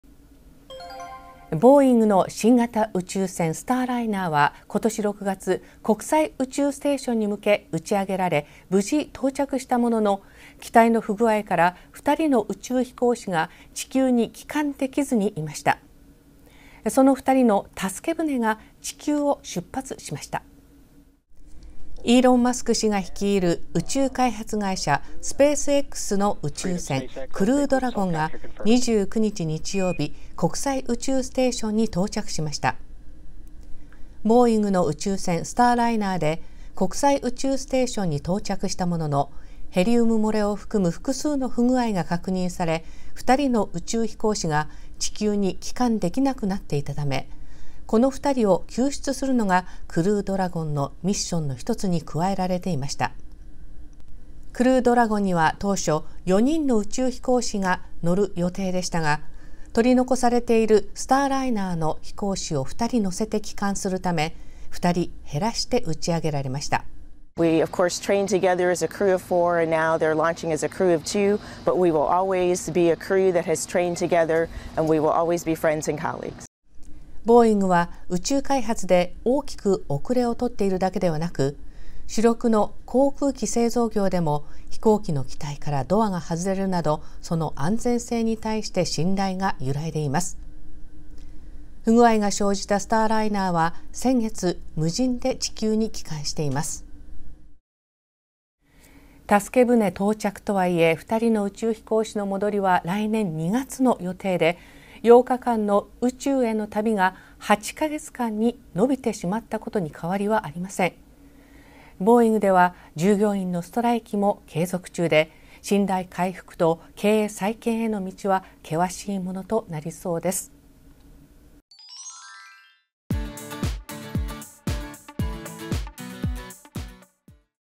居残り宇宙飛行士救出ミッション スペースX帰還時に同乗
ボーイングの新型宇宙船スターライナーは今年６月国際宇宙ステーションにむけ打ち上げられ無事到着したものの機体の不具合から２人の宇宙飛行士が地球に帰還できずにいました。その２人の助け舟が地球を出発しました。

イーロン・マスク氏が率いる宇宙開発会社スペースXの宇宙船クルードラゴンが２９日（日曜日）国際宇宙ステーションに到着しました。ボーイングの宇宙船スターライナーで国際宇宙ステーションに到着したものヘリウム漏れを含む複数の不具合が確認され２人の宇宙飛行士が地球に帰還できなくなっていたためこの２人を救出するのがクルードラゴンのミッションの一つに加えられていました。クルードラゴンには当初４人の宇宙飛行士が乗る予定でしたが取り残されているスターライナーの飛行士を２人乗せて帰還するため２人減らして打ち上げられました。

クルードラゴンの宇宙飛行士
「４人のクルーとして一緒にトレーニングをしてきました搭乗できるのは２人になってしまいましたが私たち４人は仲間であり同志であり続けます

ボーイングは宇宙開発で大きく遅れをとっているだけではなく、主力の航空機製造業でも、飛行中の機体からドアが外れるなど航空機の安全性に対しても信頼が揺らいでいます。不具合が生じたスターライナーは先月（９／６）、無人で地球に帰還しています。

助け舟の到着とはいえ２人の飛行士の戻りは来年２月の予定で８日間の宇宙への旅が８ヶ月に伸びてしまったことに変わりはありません。ボーイングでは従業員のストライキも継続中で信頼回復と経営再建への道は険しいものとなりそうです。

全米での放送地域・チャンネル情報は